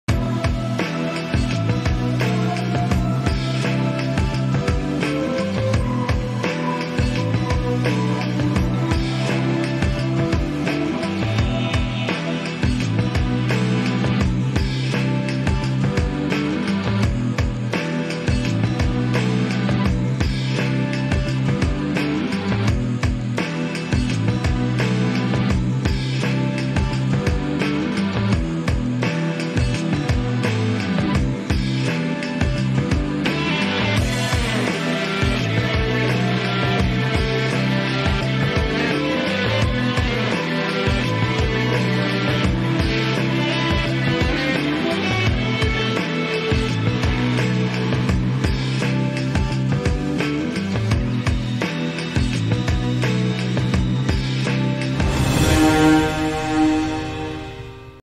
Raffi-Nagita ke Nikahan Kaesang Naik Pesawat Pribadi
Pasangan Raffi Ahmad dan Nagita Slavina mengaku senang bisa hadir dalam acara akad nikah pernikahan Kaesang Pangarep dan Erina Gudono yang digelar di Pendopo Agung Royal Ambarrukmo Sleman Yogyakarta, Sabtu (10/12/2022).